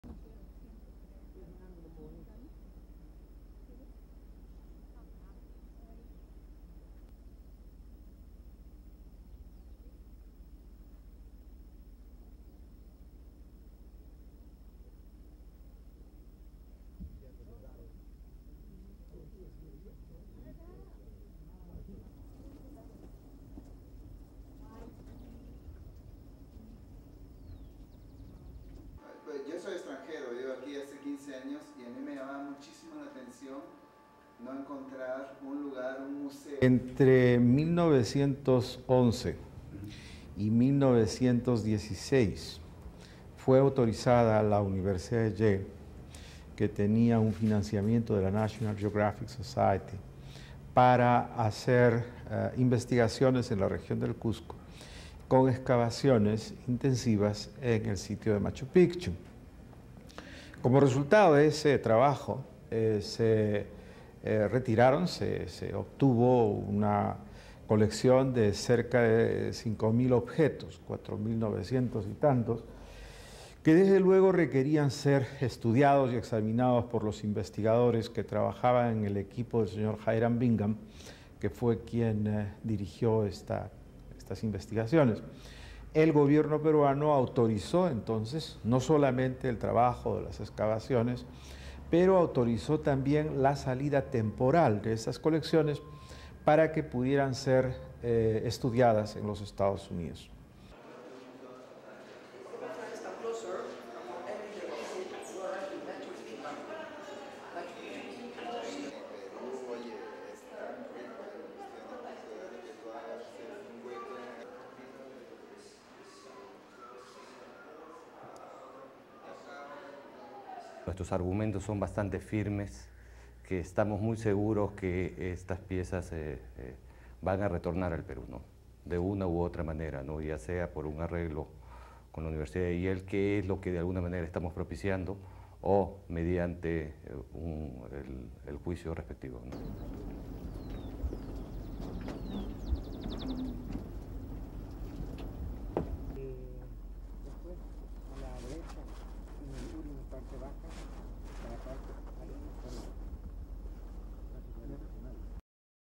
Peru to sue Yale unless ancient relics returned
FILE 2000 - AP Television
1. Various of Machu Picchu
02 December, 2005 - Lima, Peru
AP Television
2. Mid view of Luis Lumbreras
3. SOUNDBITE (Spanish) Luis Lumbreras, Head of the National Institute of Culture:
"Sometime between 1911 and 1926, Yale University was authorised to perform archaeological digs in the region of Cuzco with a National Geographic Society grant. Many of the digs took place on the grounds of the Machu Picchu, which resulted in the collection of nearly 5,000 objects. These pieces needed to be studied and examined by investigators who worked on in the field with  Hiram Bingham, who was leading the excavations. At this time, not only did the Peruvian government authorise the digs, they also allowed the pieces to temporarily be taken out of the country so that they could be studied temporarily."
4. Pan of archaeological exhibit in museum
5. Various of archaeological pieces
6. SOUNDBITE (Spanish) Manuel Soares, Minister of Cultural Affairs:
"Our arguments are quite solid. We are very certain these pieces (which are in the United States) will return to Peru, one way or another. Whether it is through some sort of agreement with Yale University, which we would prefer, or through legal means."
7. Various of Machu Picchu
STORYLINE:
Peru has formally warned  top U.S university,Yale, it will be sued if relics taken from Machu Picchu, by  U.S. explorer Hiram Bingham nearly a century ago, are not returned, the Foreign Ministry said on Friday.
The threat was delivered to Yale in a letter written by Peru's ambassador to the United States, Eduardo Ferrero, according to the statement.
The letter's date was not specified.
Luis Guillermo Lumbreras, chief of Peru's National Institute of Culture, told AP in an interview on Wednesday that Peru was preparing for a court battle to retrieve nearly 5-thousand artifacts, including mummies, ceramics and human bones, excavated by Bingham during three expeditions in 1911, 1912 and 1914.
Lumbreras said former President Augusto B. Leguia gave Bingham "permission to temporarily export the objects for scientific ends," for a period of one year.
Lumbreras said that was later was extended by 18-months and  the items should have been returned in 1916.
Cultural Affairs Minister, Manuel Soares said Peru's arguments for retrieving the artifacts are "quite solid".
Soares said he is certain the pieces will return to Peru, whether it is through some sort of agreement with Yale University, which  would be preferable, or through legal means.
The ruins at Machu Picchu, located on a craggy mountaintop above a lush valley 500 kilometres (310 miles) south-east of Lima, are Peru's top tourist attraction.